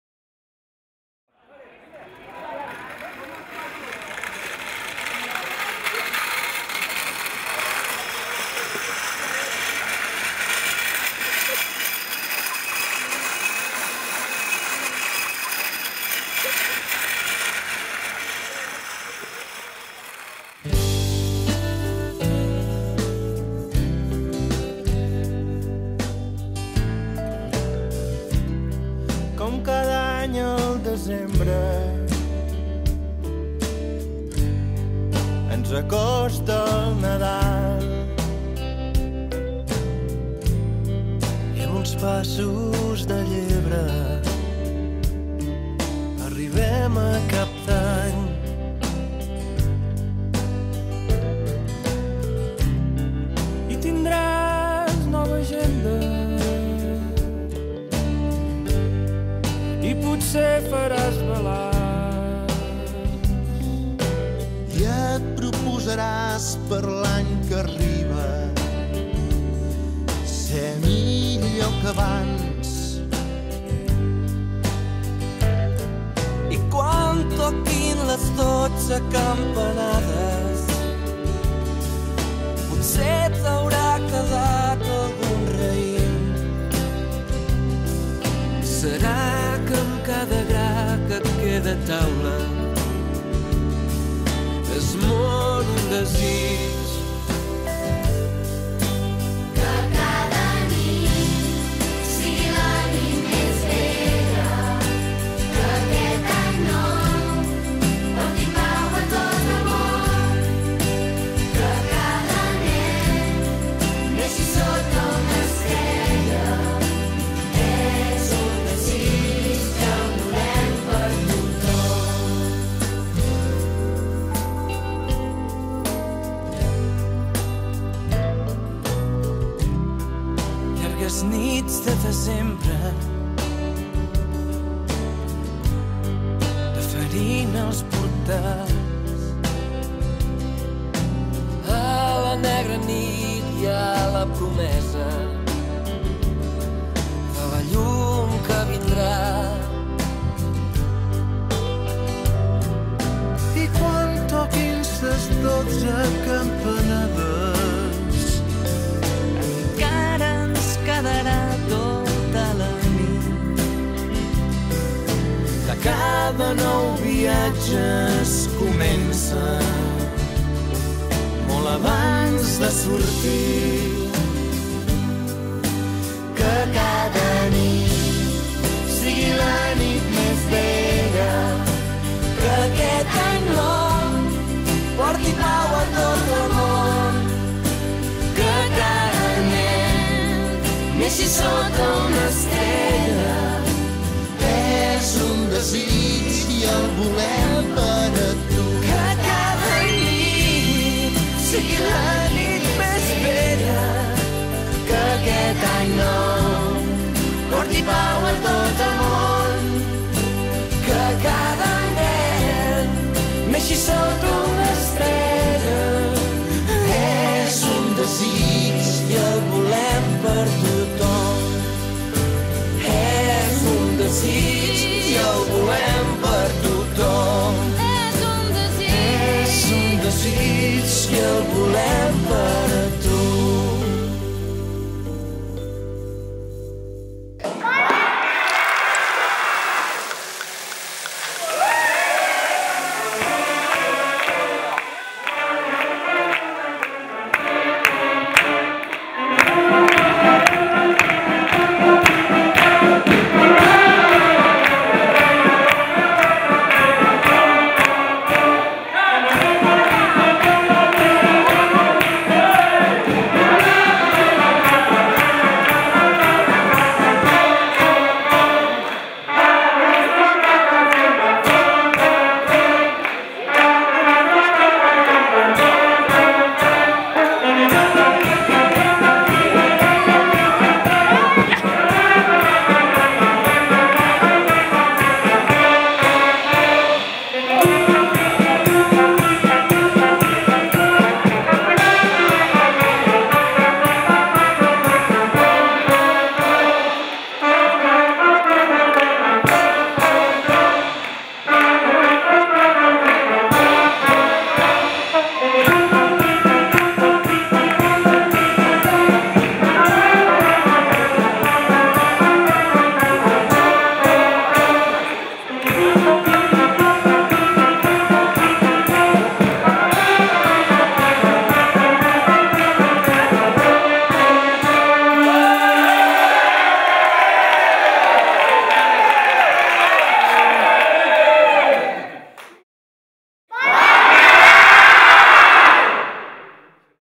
Felicitació de Nadal
El Centre Cultural Calisay i tots els seus habitants, gegants, pastors i pastores, la Verge Maria, dimonis i follets, patges i reis felicitem les festes a tots els arenyencs i arenyenques amb aquest video que hem enregistrat.

Bon Nadal!!!